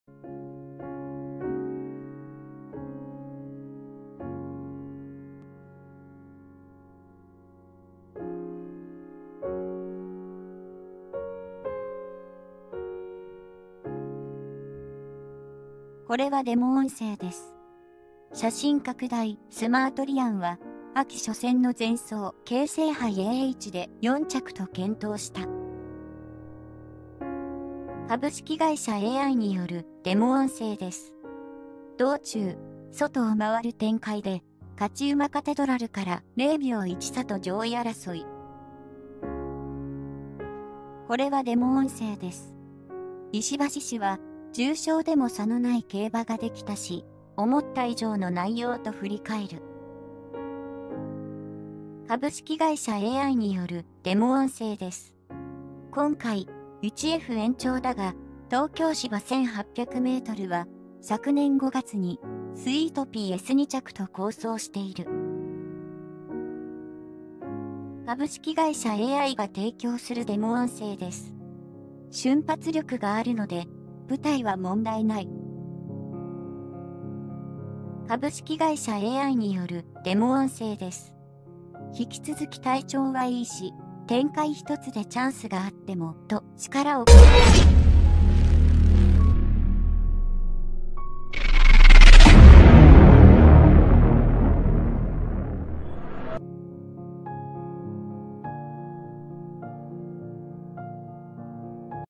【府中牝馬S】スマートリアン 展開かみ合えば、石橋師「瞬発力があるので舞台は問題なし」|Top News
【府中牝馬S】スマートリアン 展開かみ合えば、石橋師「瞬発力があるので舞台は問題なし」|Top News 写真拡大 スマートリアンは秋初戦の前走・京成杯AHで4着と健闘した。道中、外々を回る展開で勝ち馬カテドラルから0秒1差と上位争い。石橋師は「重賞でも差のない競馬ができたし、思った以上の内容」と振り返る。今回、1F延長だが東京芝1800メートルは昨年5月にスイートピーS2着と好走している